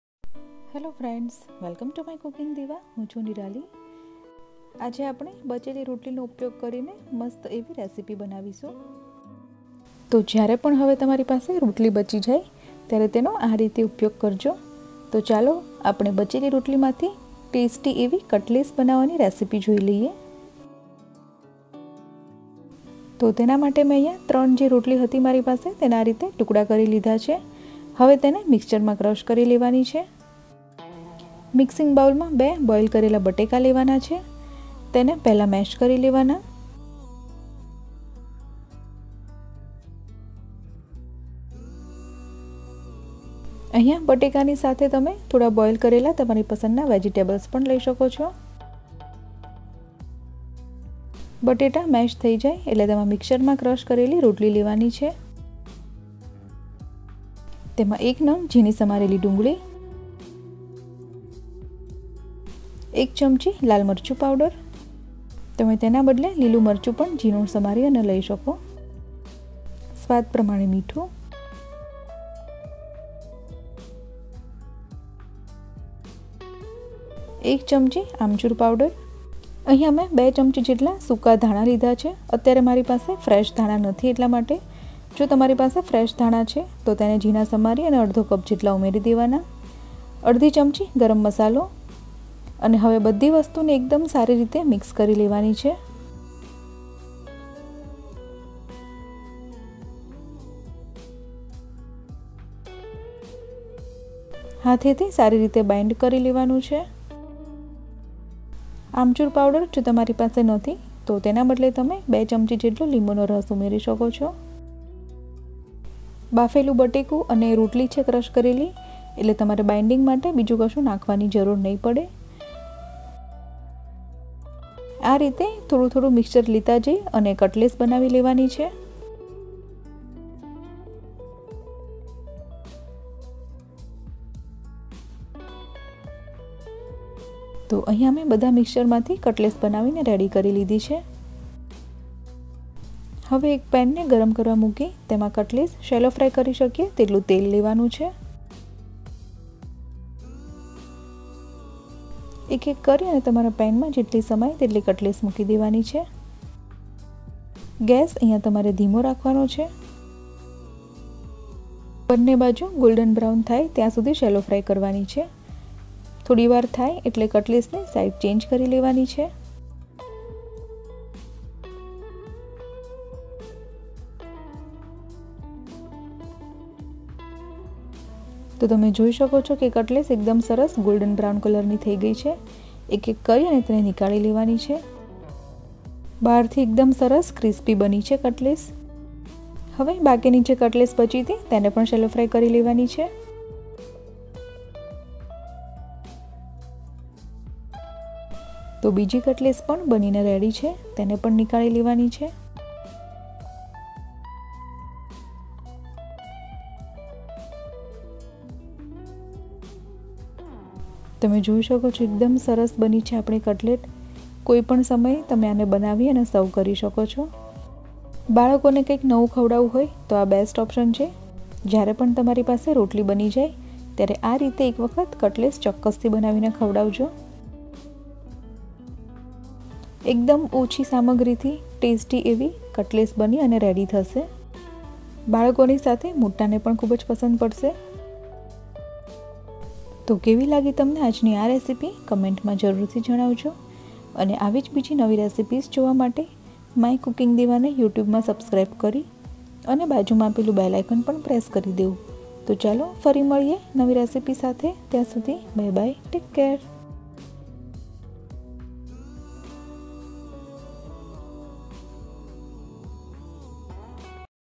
Leftover Chapati Cutlet/vadheli rotli no use gujrati/vadheli rotli no nasto/leftover roti recipe
3 leftover chapati
2 boiled potato
1 chopped onion
1 tsp red chili powder
1 tsp aamchur powder
salt to taste
1/2 tsp garam masala
2 tsp dry coriander OR 1/2 cup chopped fresh coriander
oil to shallow fry

see this very easy to make Left over chapati cutlet recipe ,  you can serve it anytime with tomato ketchup or chutney.